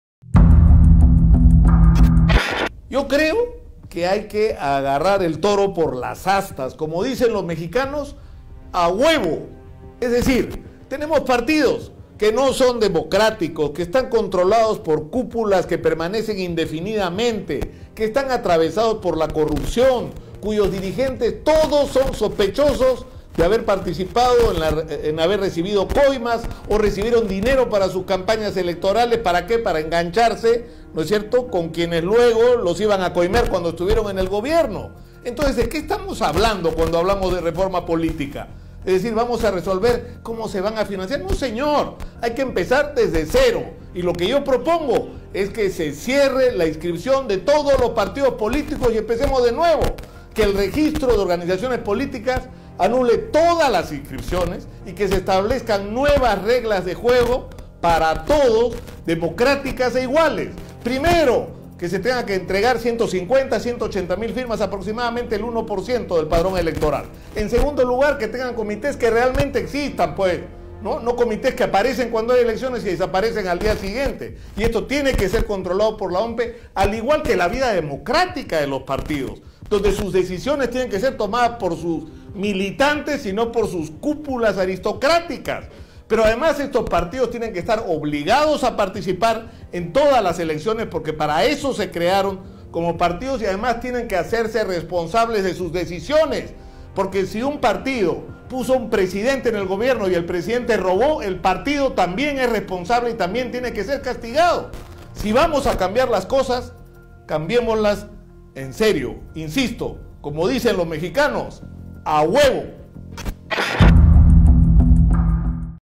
¡Que se cierren los partidos y empecemos de cero!
¡Que se cierren los partidos y empecemos de cero! No es posible que si un presidente entra al gobierno a robar, el partido no se haga cargo.